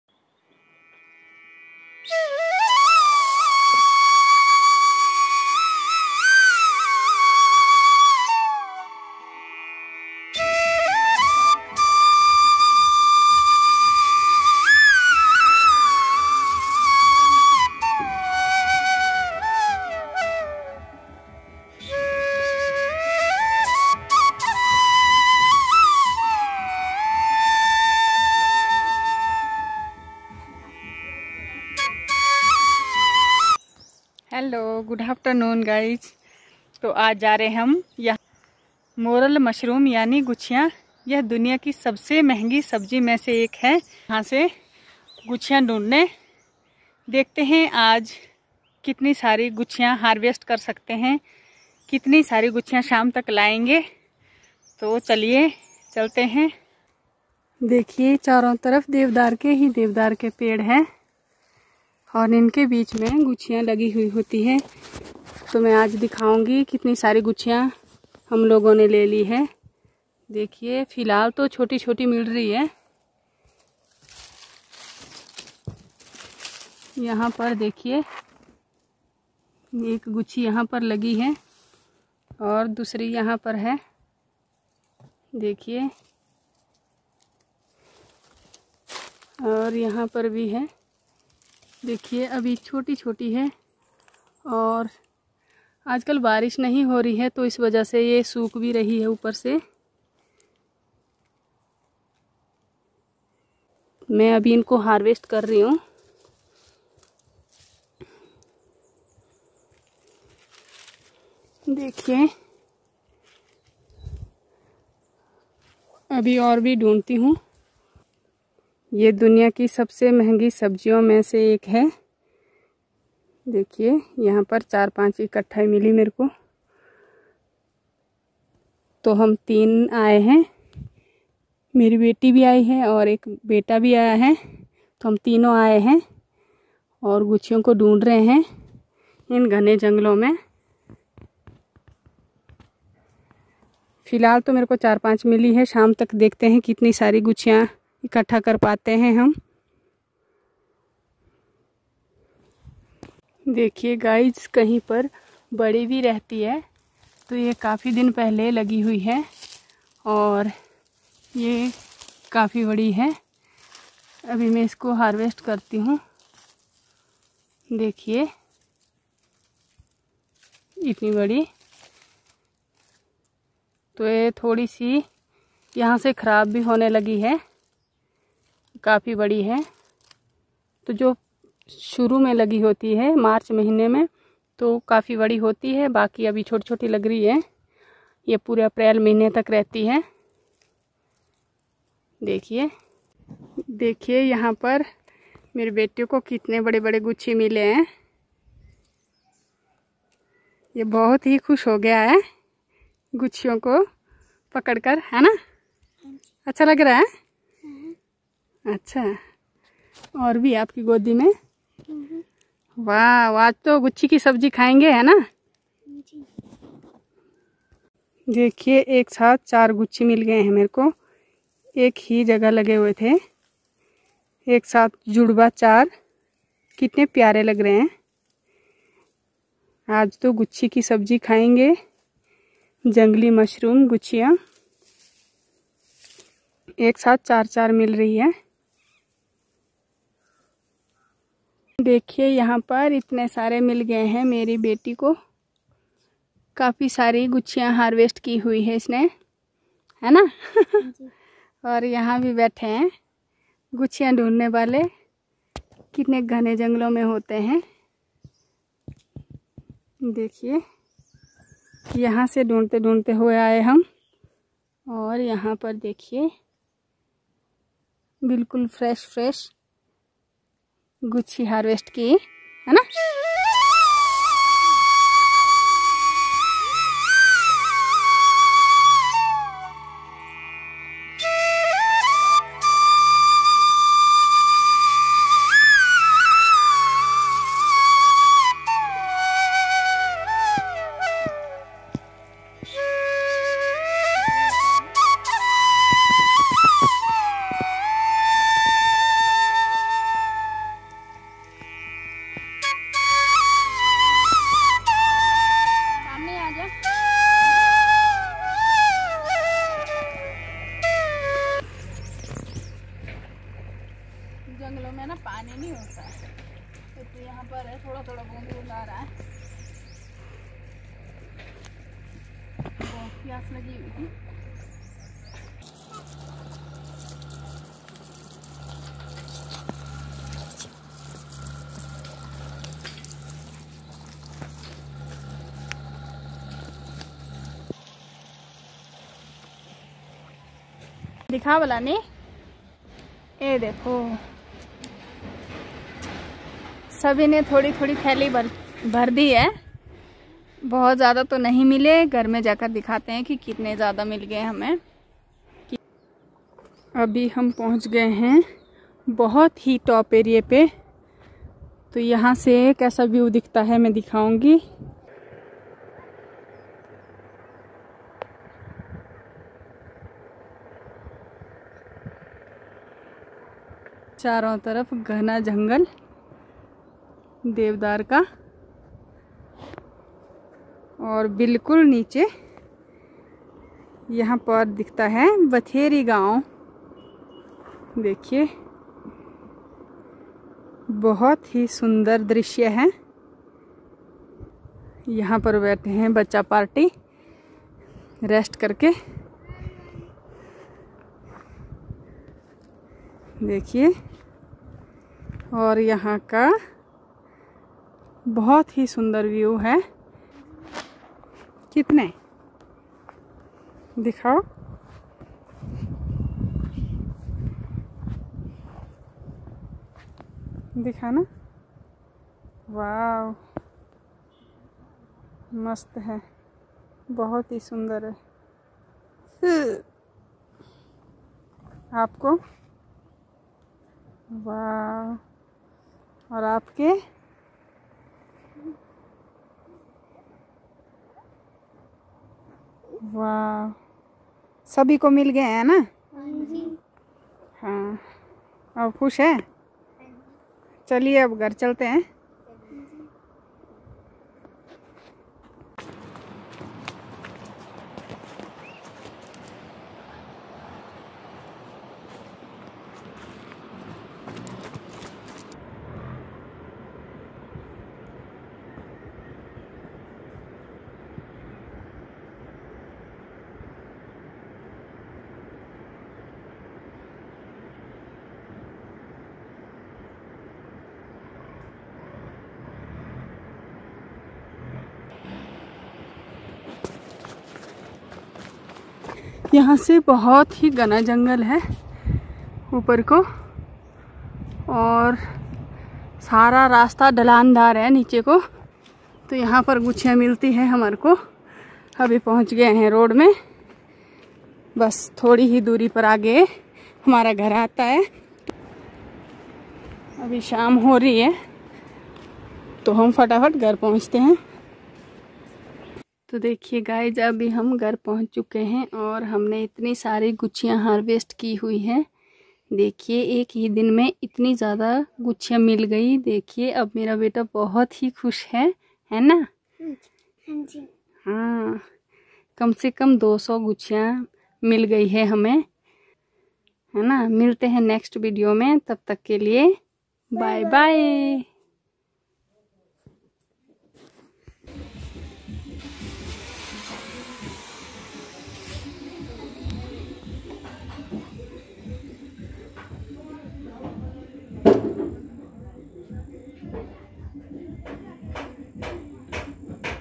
Morel mushroom /दुनिया की सबसे महंगी सब्जी गुच्छियां ll
इस विडियो में दुनिया की सबसे महंगी सब्जी को दिखाया गया है जो कि पहाड़ो में होती है। यह सब्जी दुनिया की सबसे महंगी सब्जी गुच्छियां मानी जाती है। यह सिर्फ पहाड़ी क्षेत्रों में ही पाई जाती है। नेचुरल एंड ऑर्गेनिक है। यह सब्जी कई सारे रोगों से बचाती है। इस सब्जी का सेवन जरूर करें।